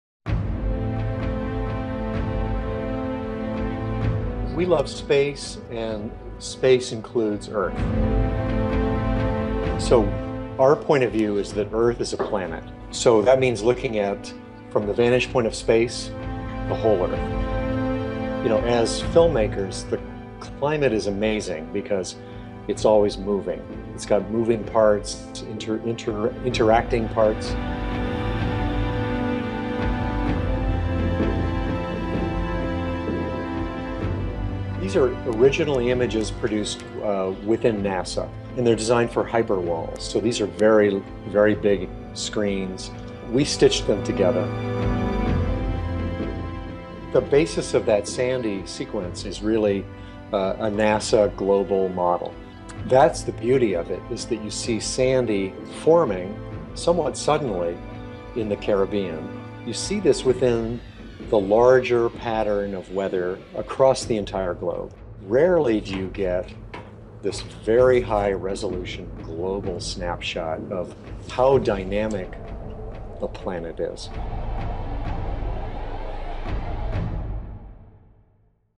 Earth Beyond HD
A new ultra high definition video of Earth has been released, showing high resolution computer generated images of weather pattern, including the formation of Superstorm Sandy.